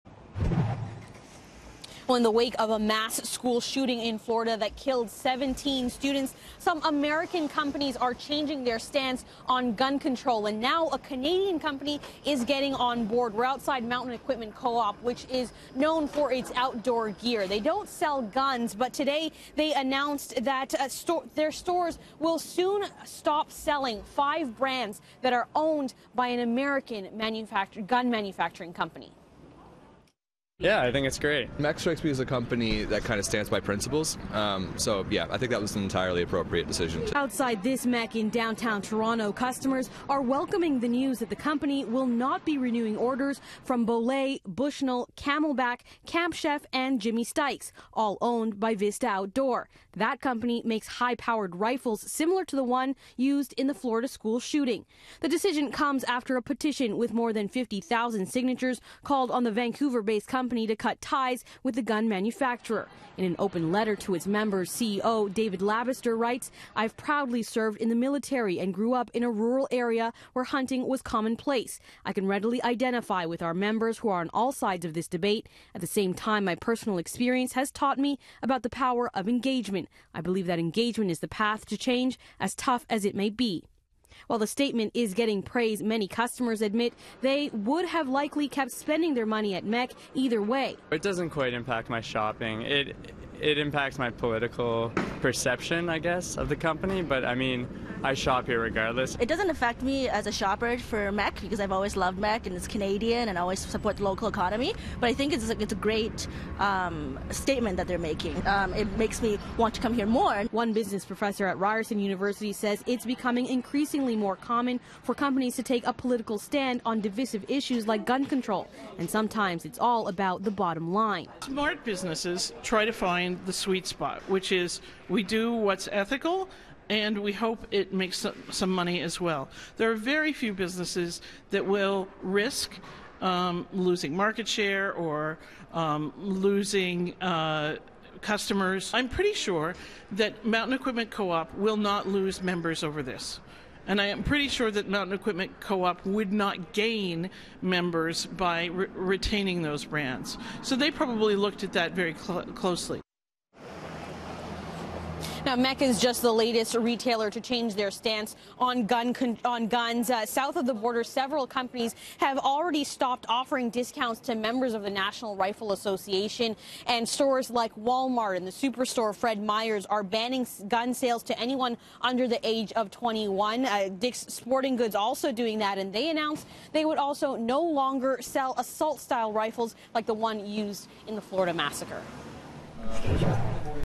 MEC to stop carrying brands tied to U.S. gun-maker
By stopping the sale of brands tied to gun manufacturing, Mountain Equipment Co-op says it can play a role in advancing discussions around gun control, in the wake of a mass shooting in Florida.